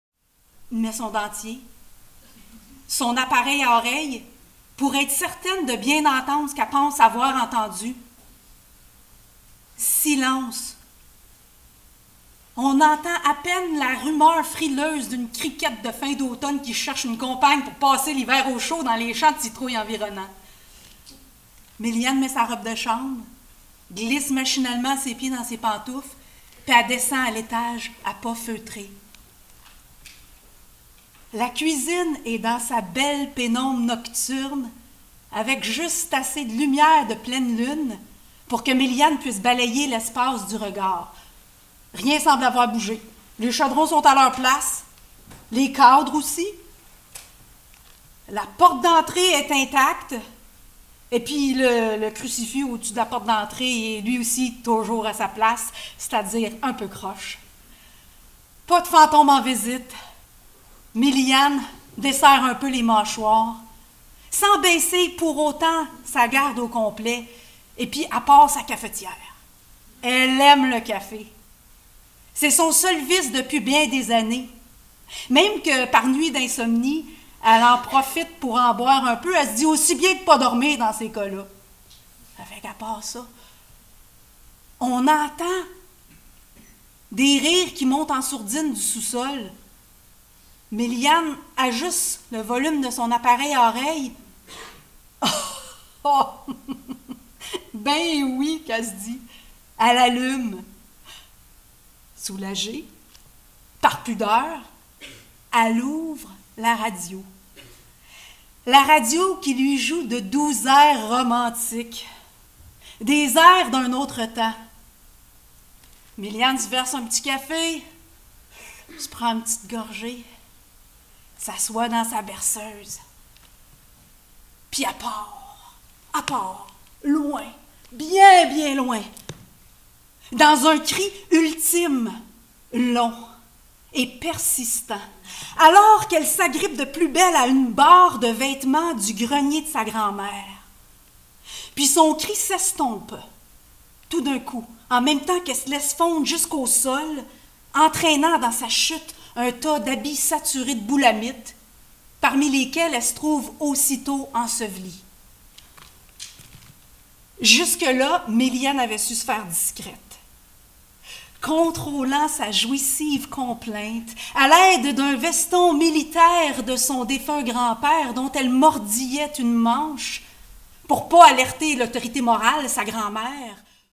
Caroline Legault - D'amour et de boules-à-mites - Marathon du conte 2013
Extrait d'un conte de création : D'amour et de boules-à-mites.

CAROLINE LEGAULT, conteuse québécoise, en spectacle lors du 12e Festival interculturel du conte du Québec • Extrait capté à Montréal à la maison de la culture Frontenac lors
 du « Marathon du conte », le 27 octobre 2013.

Captation : Jean Olivier & Jean Jauniaux
Montage : Tomi Grgivecic